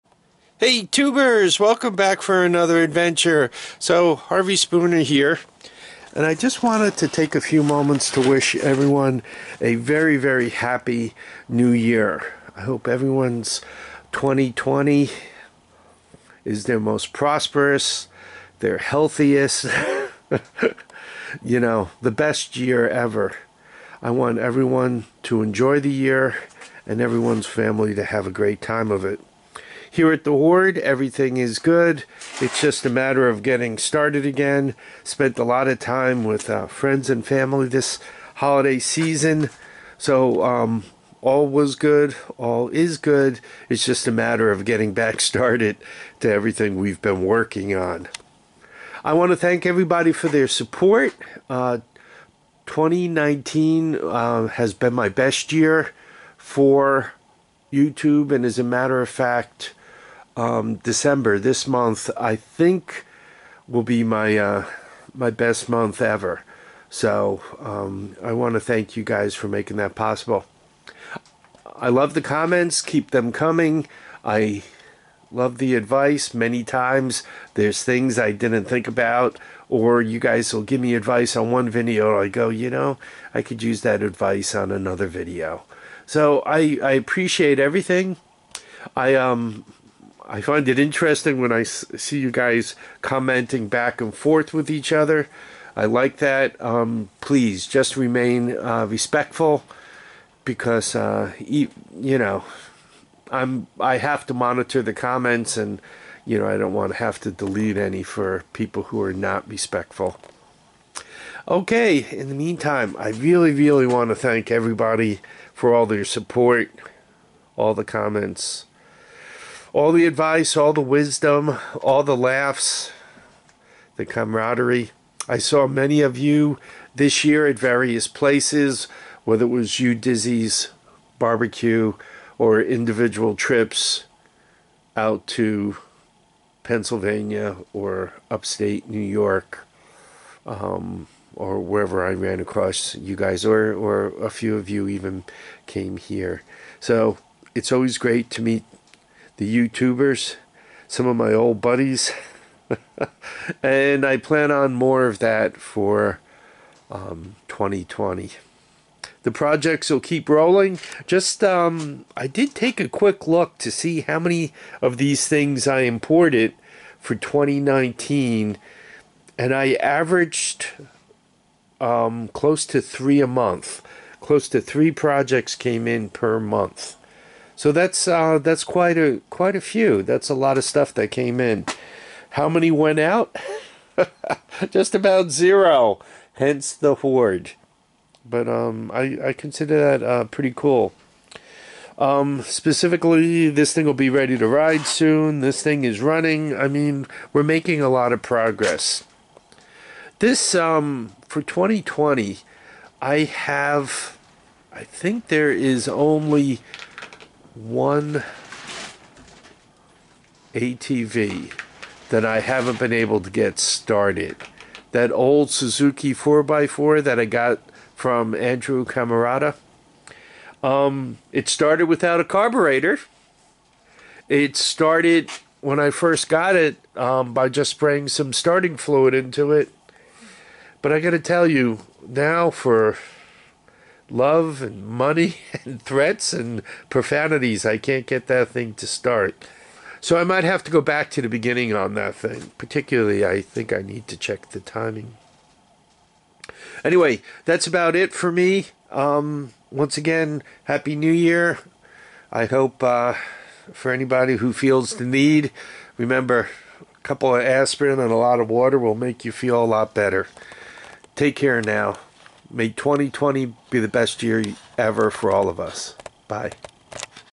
Happy New Year, My Best Wishes for 2020!
Hey Tubers, In 2019, I averaged almost 3 new projects per month, for 2020, my plans are to continue!  Thanks to all who watch my videos!!! A very special thanks to all of my subscribers and to the folks who take the time to comment. My best to everyone in 2020!